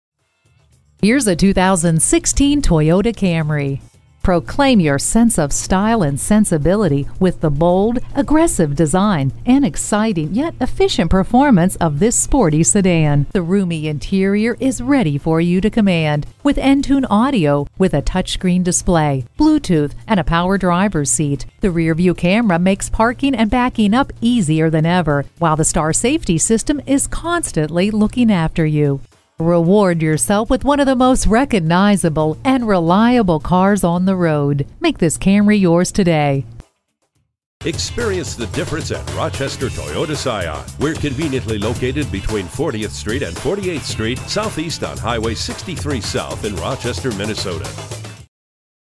2016 Toyota Camry Rochester MN Winona, MN S26356 - SOLD
Year: 2016
Make: Toyota
Model: Camry
Trim: SE
Engine: 2.5L I4 SMPI DOHC
Transmission: 6-Speed Automatic
Color: Red
Interior: Black
Stock #: S26356
VIN: 4T1BF1FK2GU528045
 Price includes the following rebates and incentives from Toyota: $1,250 - TMS Customer Cash. Exp. 11/02

Address:
4900 Highway 52 N